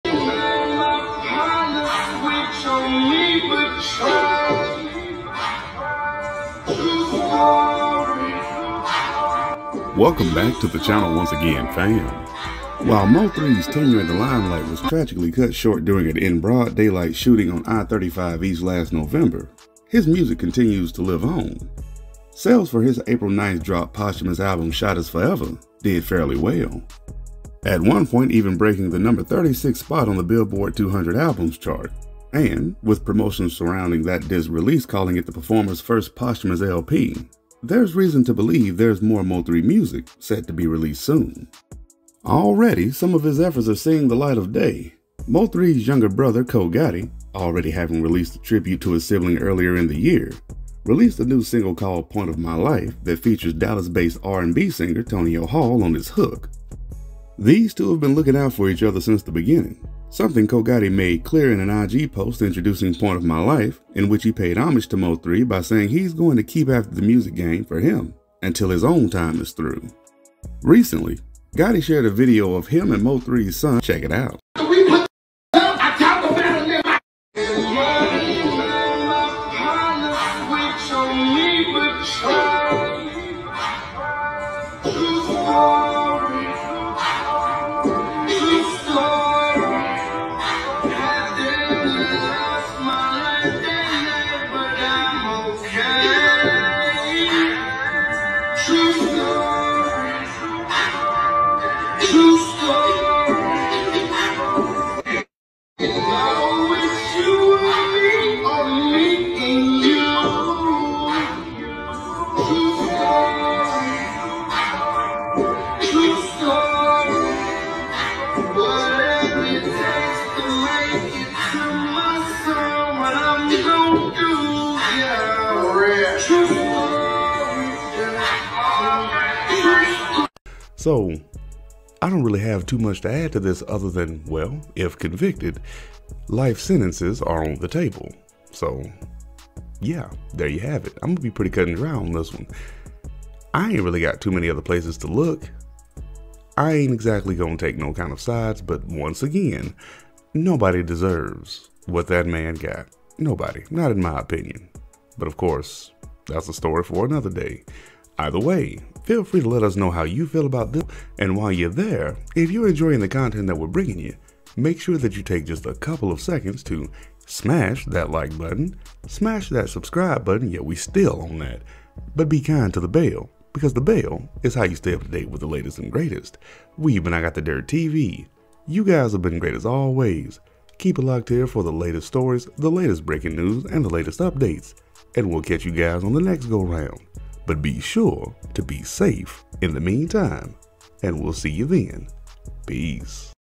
Mo3’s Son Becomes Emotional While Watching His father on Tv
Mo3 🌌